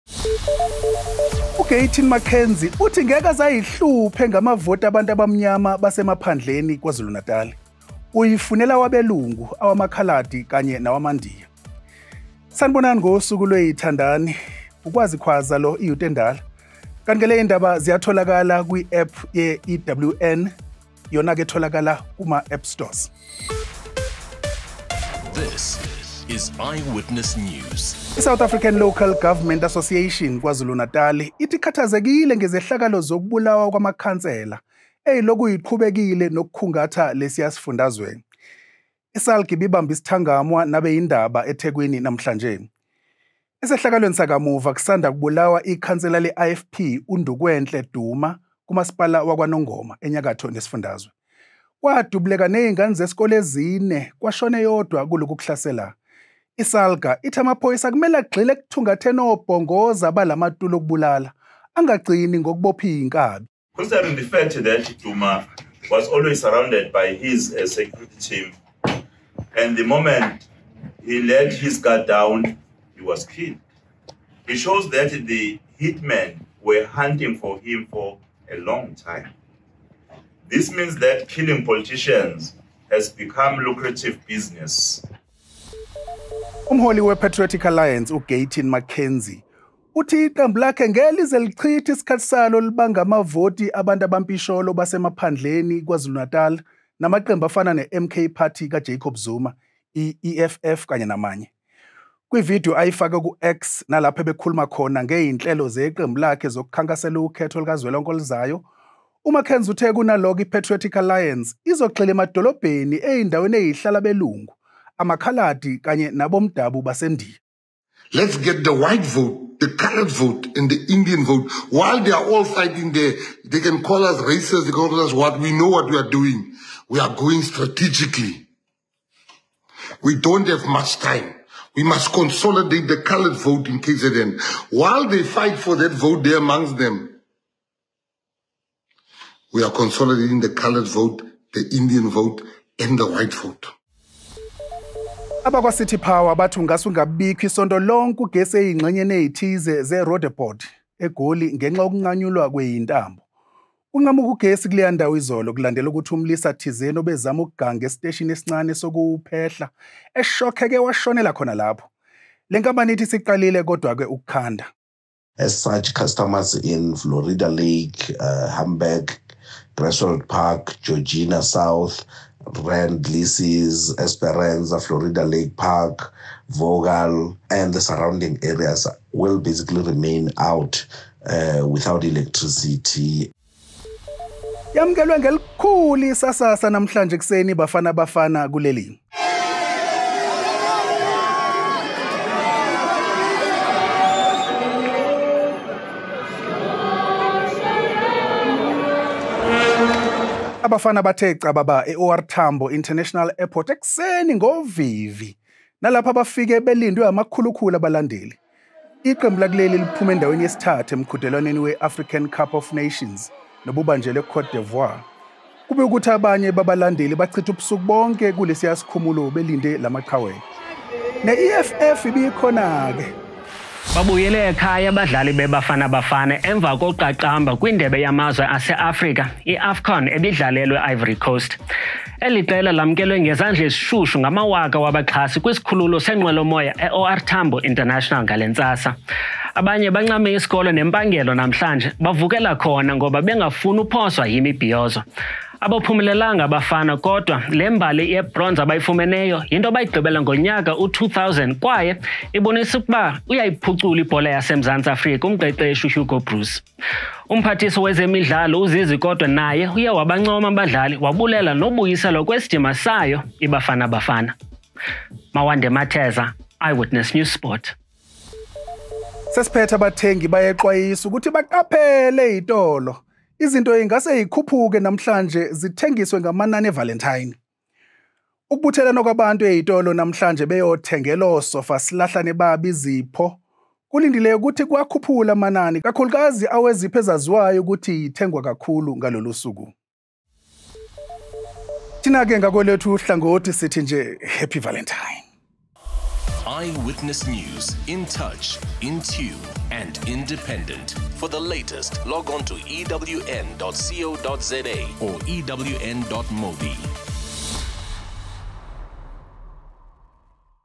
Kwezanamuhla: I-Patriotic Alliance ifuna amavoti abamhlophe eKZN, ibuyile iBafana Bafana
Izindaba zesiZulu no-Kwazi Kwaza.